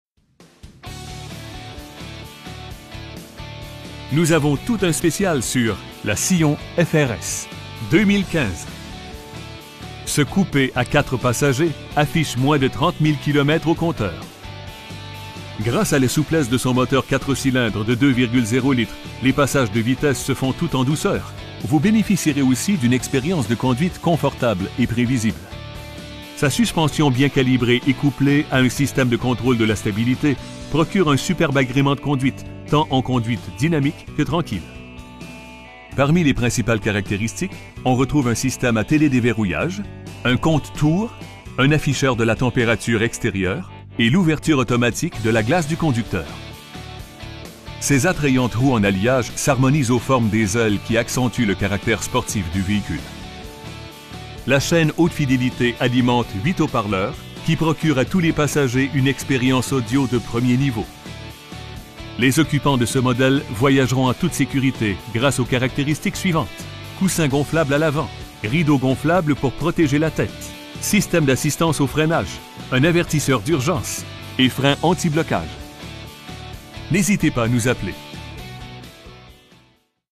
2015 Scion FR-S Manuelle 6 Vitesses
Nous avons tout un sp��cial sur la Scion FR-S 2015. Ce coup�� �� 4 passagers affiche moins de 30 000 km au compteur. Gr��ce �� la souplesse de son moteur 4 cylindres de 2,0 litres, les passages de vitesses se font tout en douceur, vous b��n��ficierez aussi d'une exp��rience de conduite confortable et pr��visible. Sa suspension bien calibr��e et coupl��e �� une syst��me de contr��le de la stabilit�� procure un superbe agr��ment de conduite, tant en conduite dynamique que tranquille. Parmi les principales caract��ristiques on retrouve un syst��me �� t��l��d��verrouillage, un compte-tours, des essuie-glaces intermittents, un ordinateur de bord, un afficheur de la temp��rature ext��rieure, des si��ges baquets �� l'avant, climatiseur, et l'ouverture automatique de la glace du conducteur. Ses attrayantes roues en alliage s'harmonisent aux formes des ailes qui accentuent le caract��re sportif du v��hicule. La cha��ne haute fid��lit�� alimente 8 haut-parleurs, qui procurent �� tous les passagers une exp��rience audio de premier niveau. Les occupants de ce mod��le voyageront en toute s��curit�� gr��ce aux caract��ristiques suivantes: coussins gonflables �� l'avant, rideaux gonflables pour prot��ger la t��te, syst��me antipatinage, syst��me d'assistance au freinage, un avertisseur d'urgence, et freins antiblocage. Notre fiert�� se manifeste �� travers l'excellent service �� la client��le que nous offrons. N'h��sitez pas �� nous appeler.